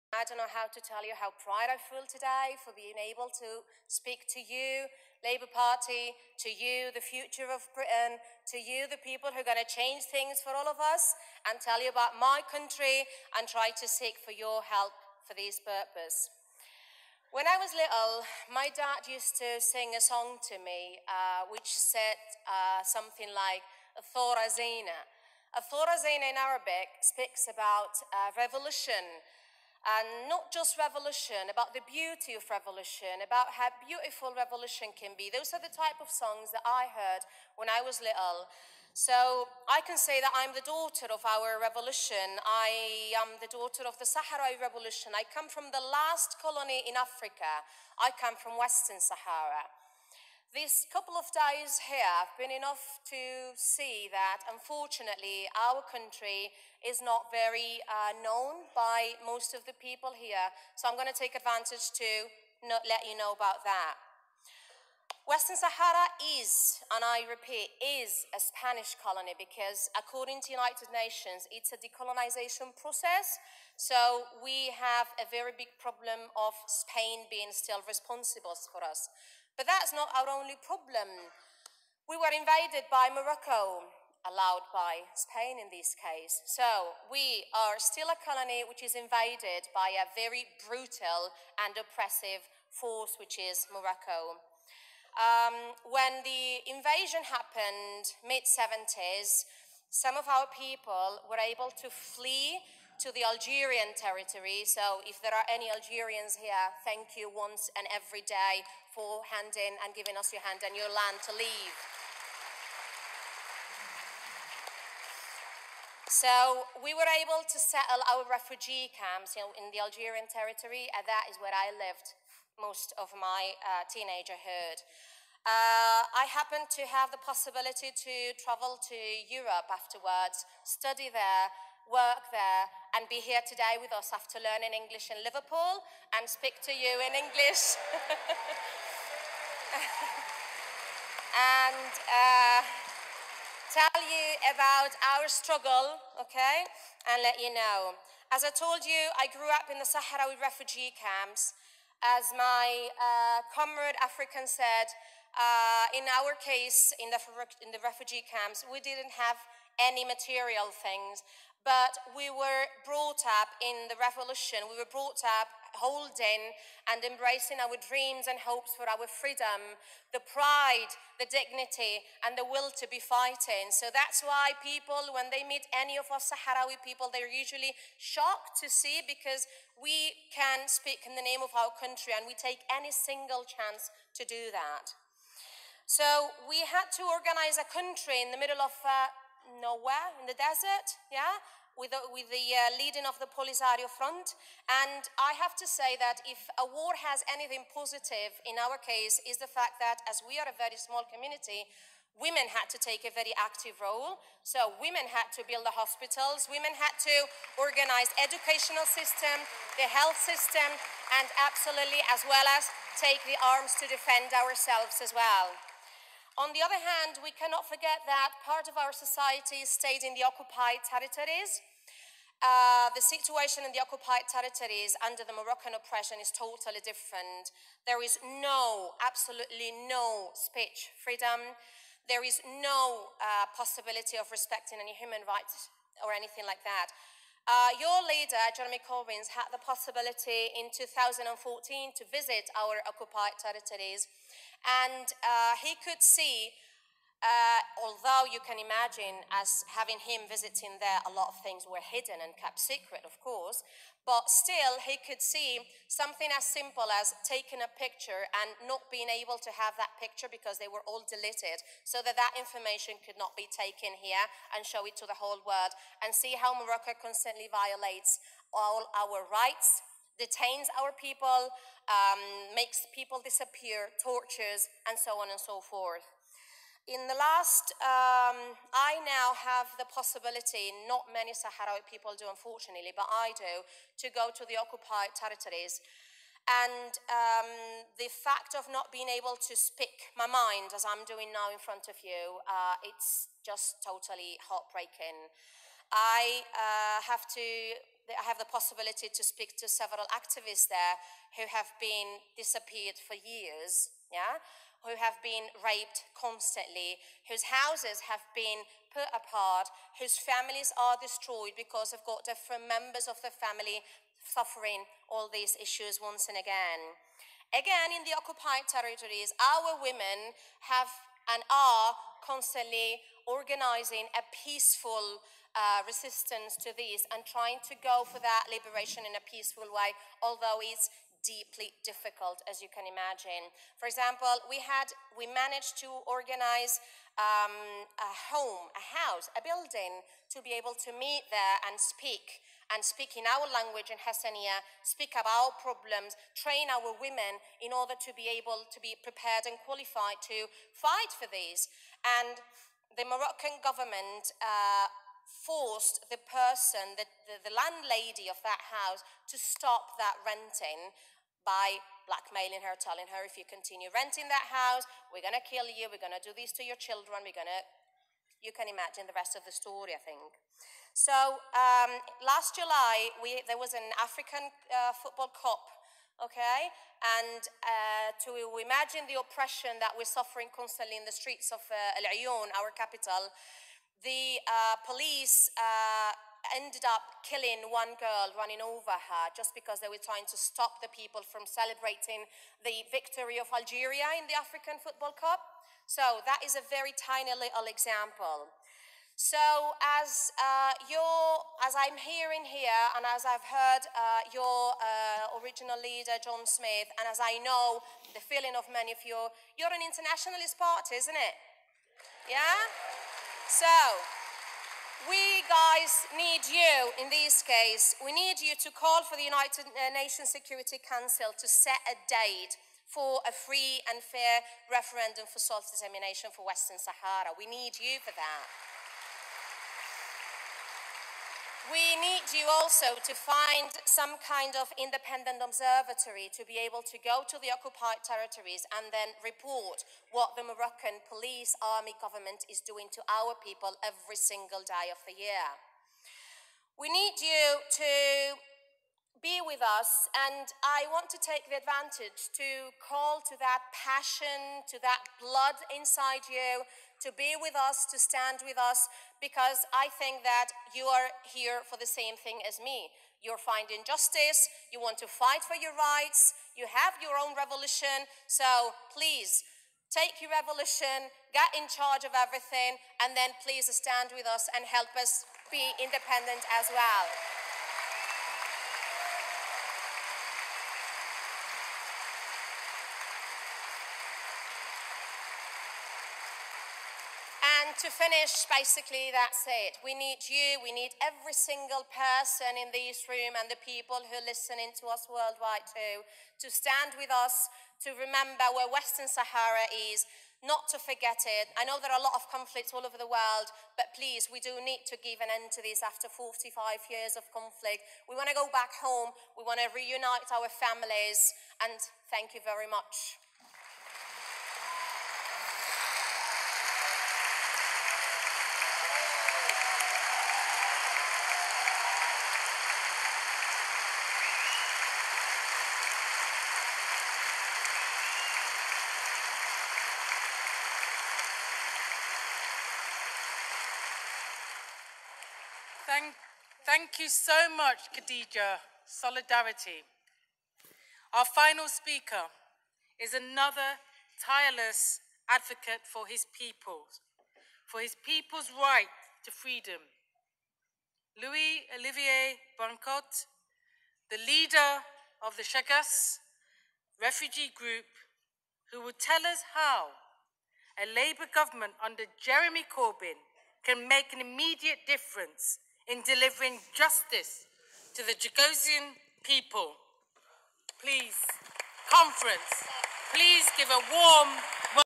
Garazi Hack Embarek
| Britainia Handiko Alderdi Laboristaren batzarrean hitz egin du.